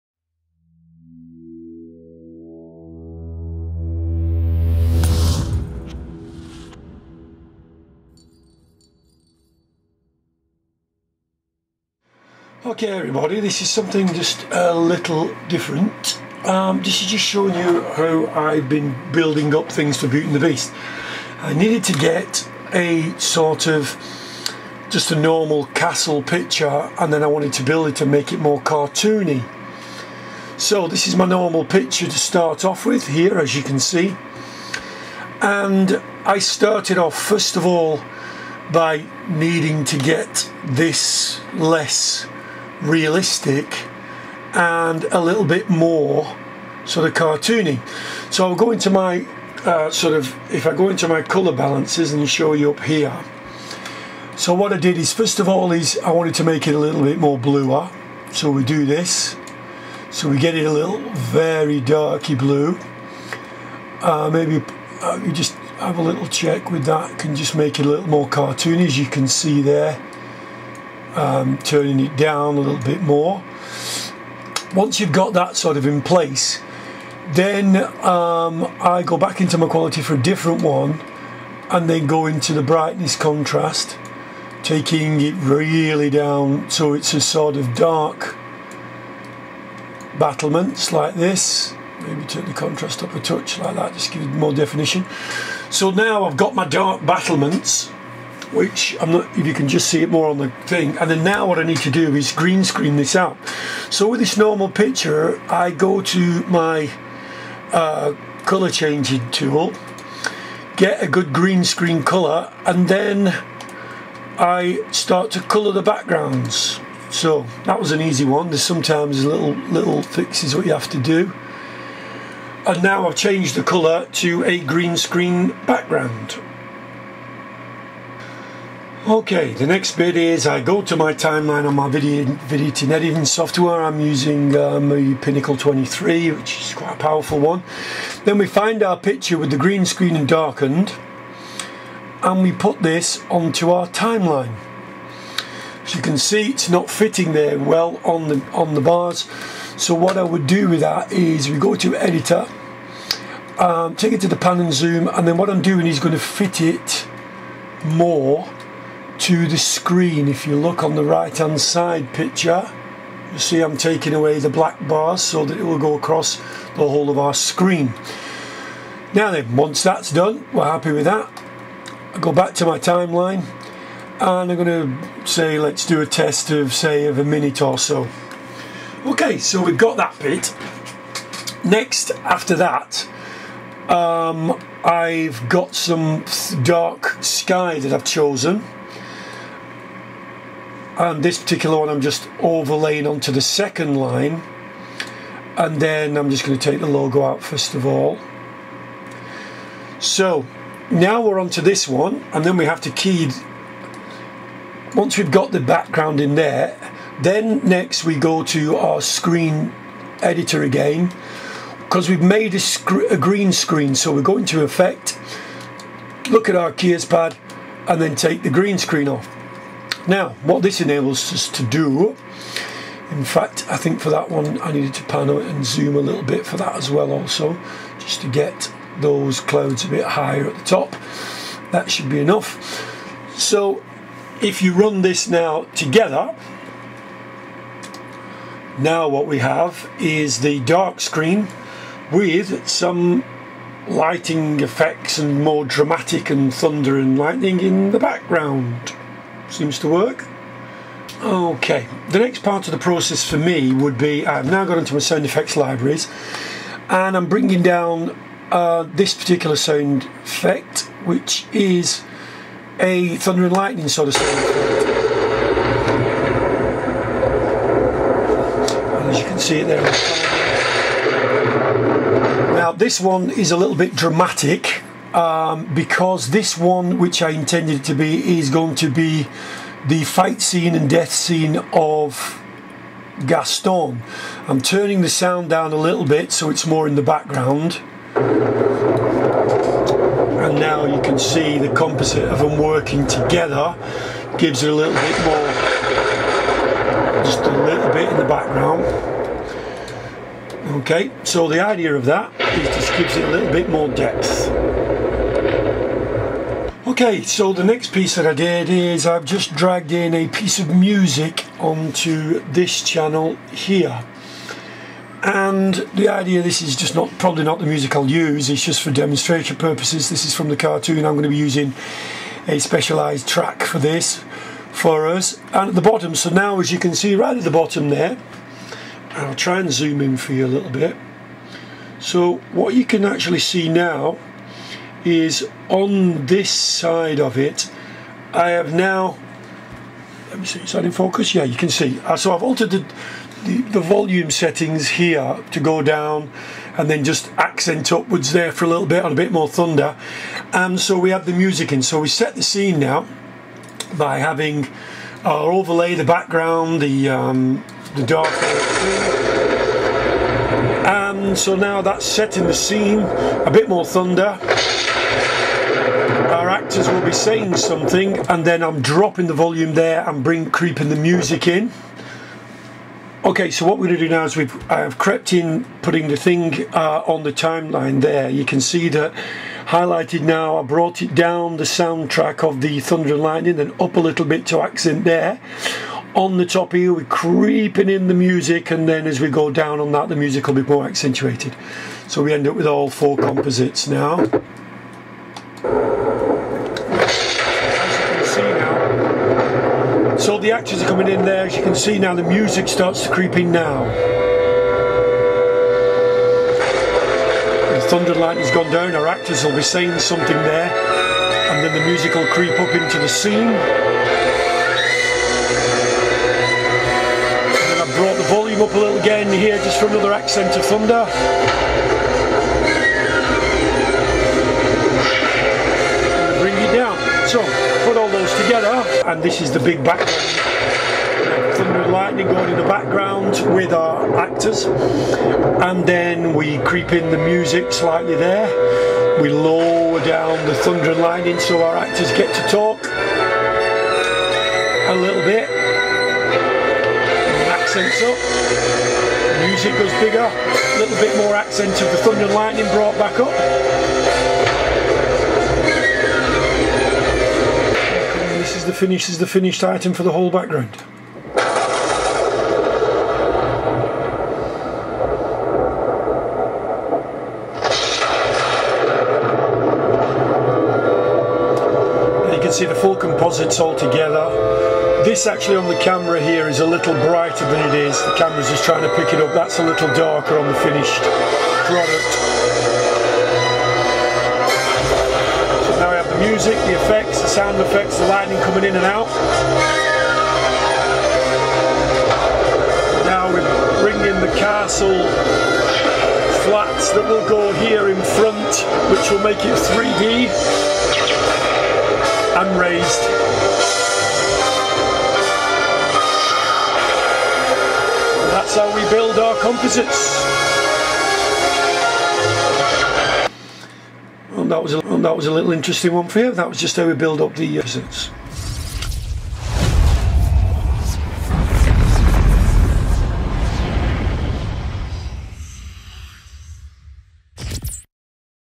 A little technical insight BUILDING A SCENE
Little inight on how to build a green screen scene for projection or stage screens